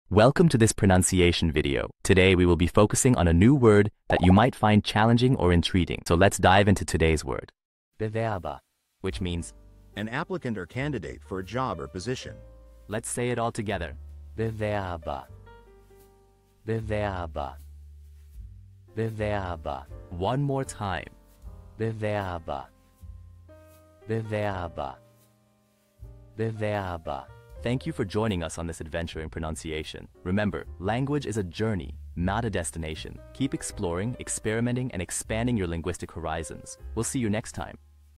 How to pronounce Bewerber
Master the Pronunciation of 'Bewerber which means applicant' - which means : To Applicant or Candidate for a job or position. 📝 with @PronunciationGerman 🎓🗣️ - [Your Guide to German]

Learn to pronounce 'Bewerber' perfectly in German with our easy and engaging tutorial by @PronunciationGerman! 📖📽️ In this comprehensive video, we provide you with native speaker examples 🗣️👂 and phonetic breakdowns 🎵 to ensure you get the pronunciation just right.

How to say Applicant in German